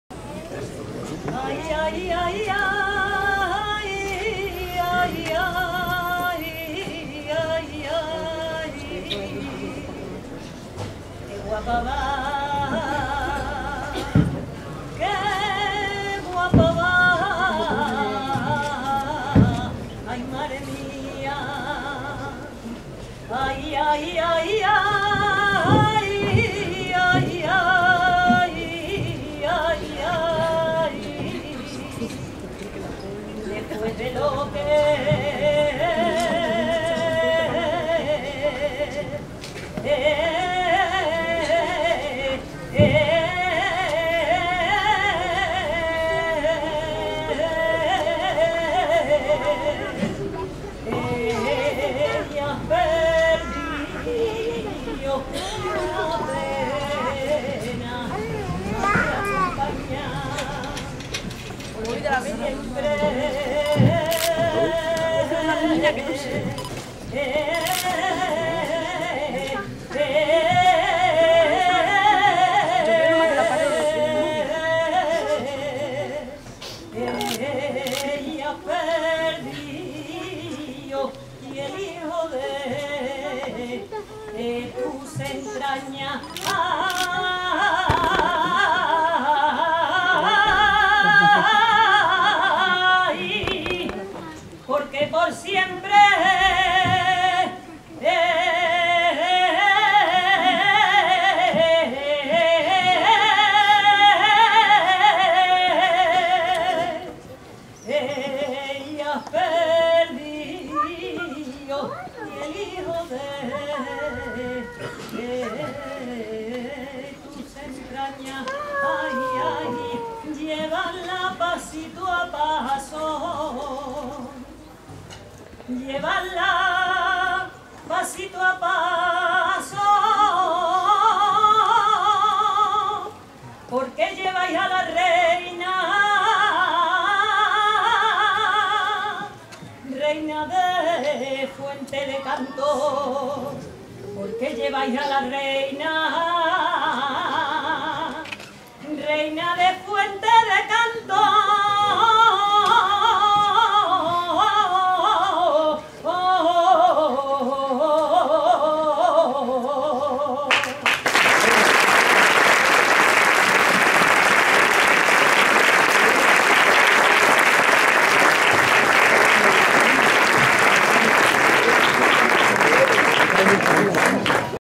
Rosario Abelaira, saeta a la Virgen de las Angustias de Fuente de Cantos
Rosario Abelaira canta su Saeta a la Virgen de las Angustias de Fuente de Cantos el jueves santo 2017